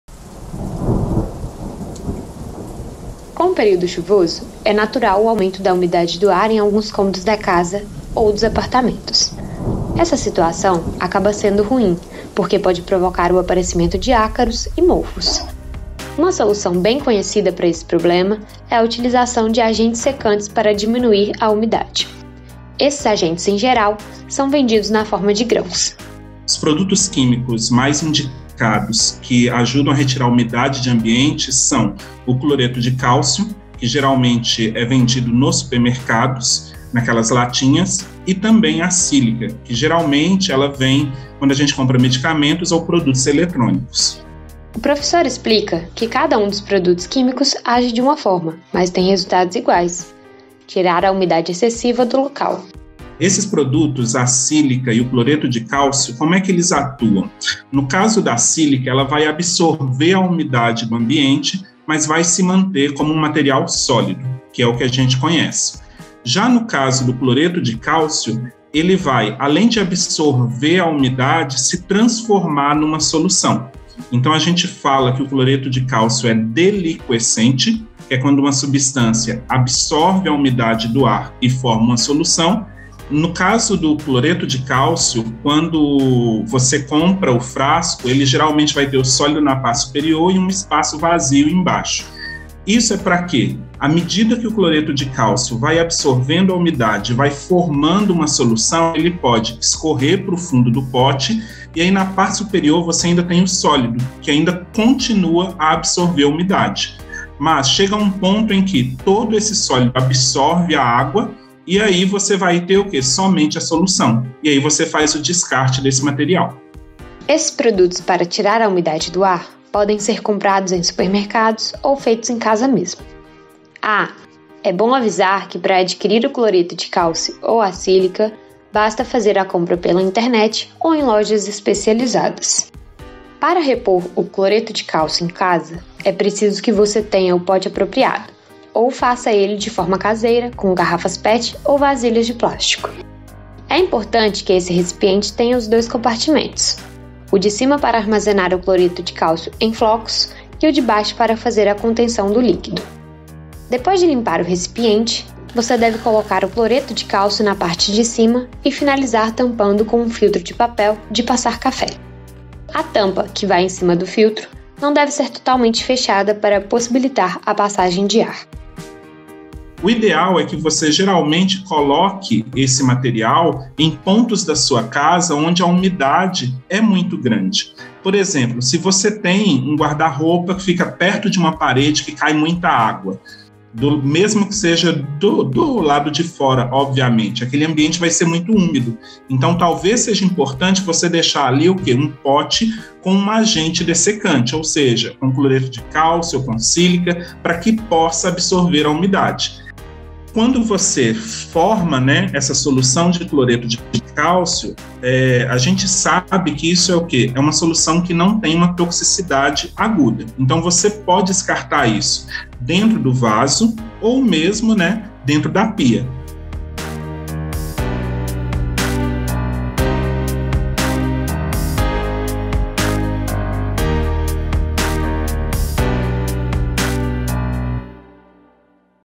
Como diminuir a umidade nas casas em tempo de chuvarada
Com a chuvadara dos últimos dias, além de tantos problemas graves nas estradas, alagamentos e enchentes muitos mineiros enfrentaram transtorno, também, com o aumento da umidade dentro das casas. E há previsão de mais chuvas pela frente.O professor do departamento de Química da UFMG, Gilson Silva, explica como ajudar a diminuir a umidade nos cômodos dos imóveis com explicações simples de como prepara produtos, que funcionam como agentes secantes os quais podem ser comprados em lojas ou feitos em casa mesmo. Vamos explicar o passo a passo para a preparação caseira.

Ficha técnica:
Produção: Bruna Gomes
Imagens: Bruna Gomes, Márcia Botelho, Renata Valentim.
Edição de imagens: Marcelo Duarte
Edição de conteúdo: Naiana Andrade
Entrevistado: Gilson Silva – Prof. Depto. De Química